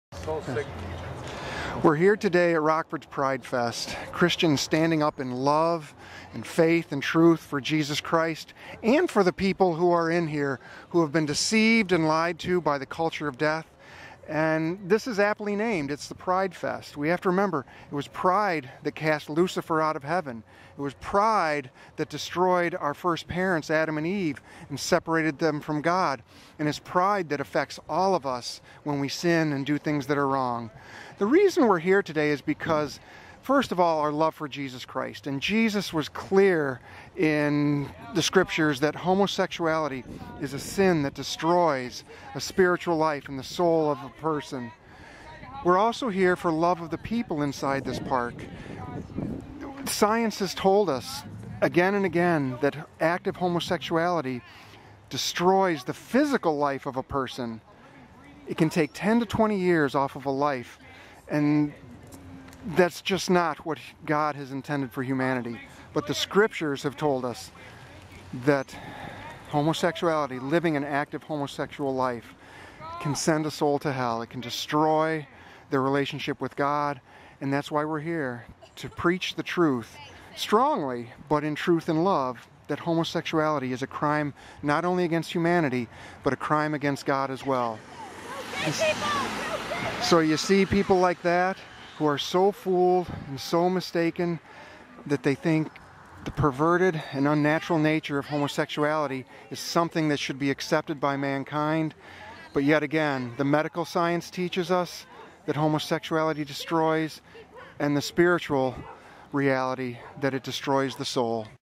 Rockford Pride Fest and the Christian Response
SORRY FOLKS - I HAD TO DISABLE THE COMMENTS BECAUSE PRIDE FEST SUPPORTERS POSTED TO MANY PERVERTED AND DISGUSTING COMMENTS.

BLESSINGS ON ALL OF YOU WHO STOOD FOR JESUS AND HIS TRUTH AND LOVE. PLEASE KEEP PRAYING FOR THOSE WHO HAVE GIVEN THEIR SOULS OVER TO PERVERSION.

Many of our neighbors in Rockford are being mentally, physically, and spiritually harmed by temptations to same sex attraction and the homosexual agenda.

In many ways our society is becoming a modern day Sodom and Gomorrah. Souls are being lost.

We cannot call ourselves Christians if we do not take a stand, in truth and love, for real Christian love and against the homosexual movement that is harming so many children of God.

The Rockford Pride Fest is about normalizing sodomy and homosexual marriage - do we love our sisters and brothers enough who are tempted by same sex attraction to take a public prayerful stand for Jesus Christ?  Will we remain silent as our country is DE-Christianized by the homosexual movement?